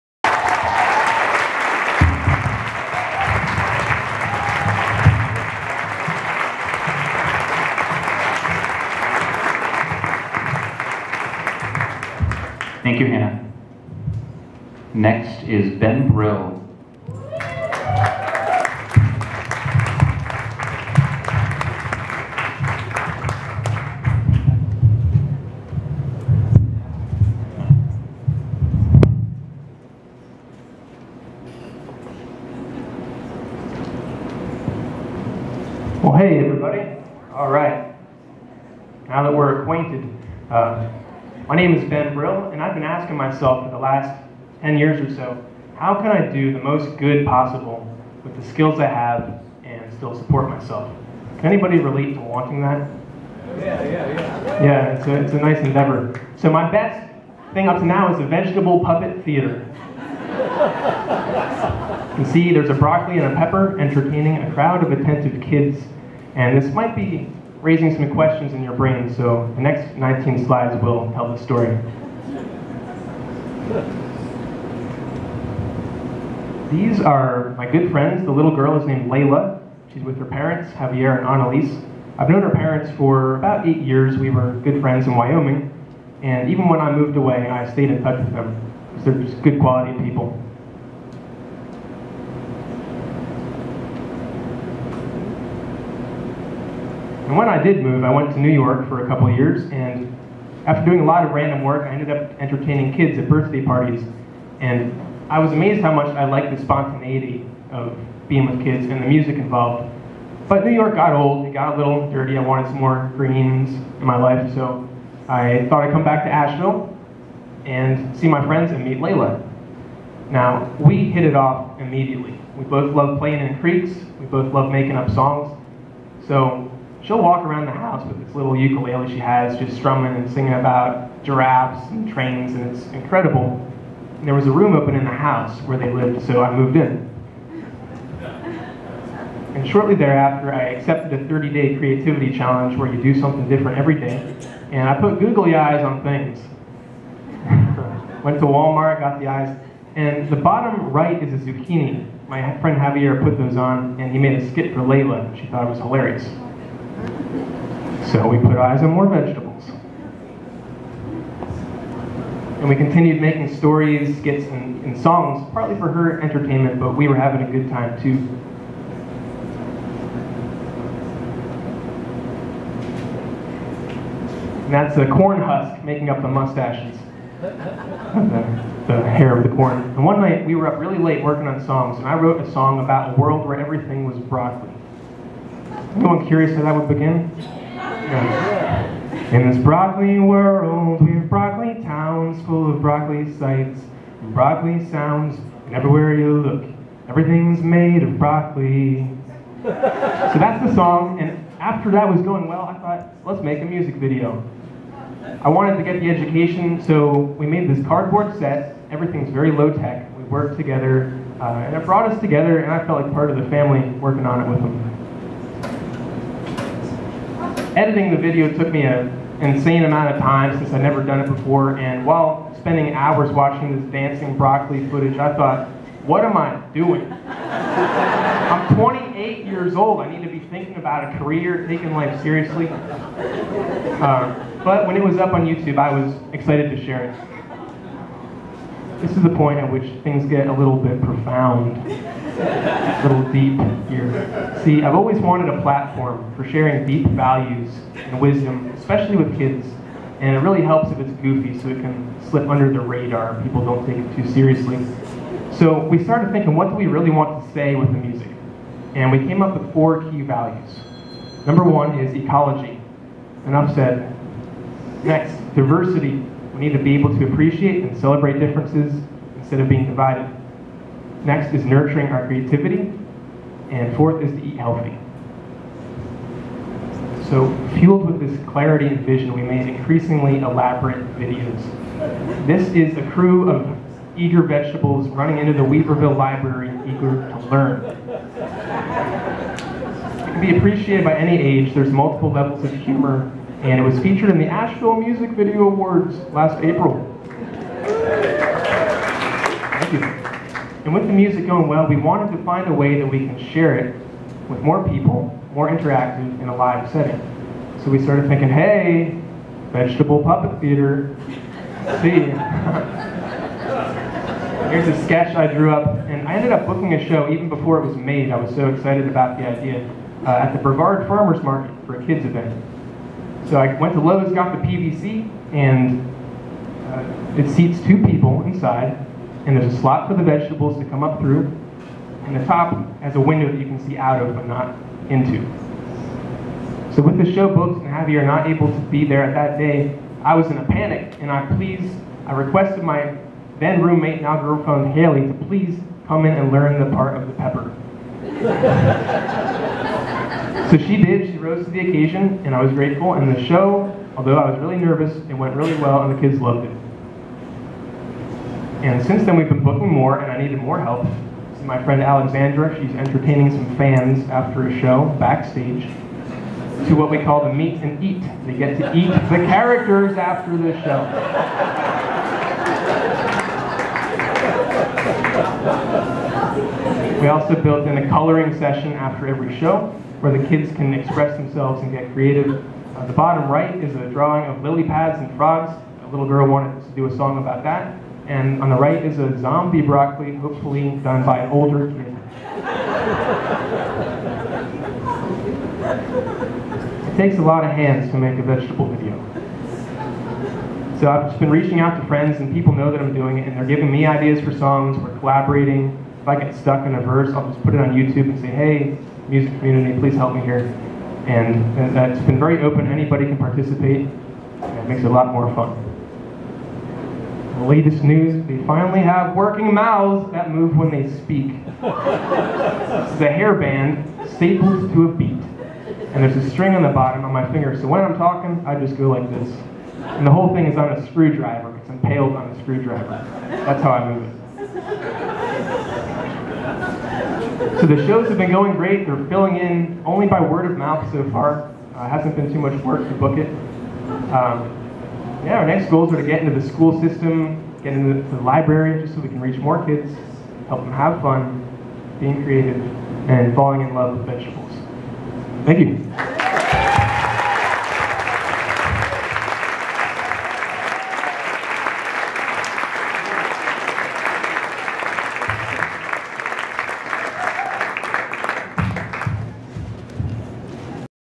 The Story of Singing Vegetables - Pechakucha talk by Ben Brill
Ben Brill explains how The Singing Vegetable Show was created. The talk is in an increasingly popular presentation format called Pechakucha where 20 slides are shown, each lasting for 20 seconds. This talk is from Asheville's Pechakucha event September 23, 2016.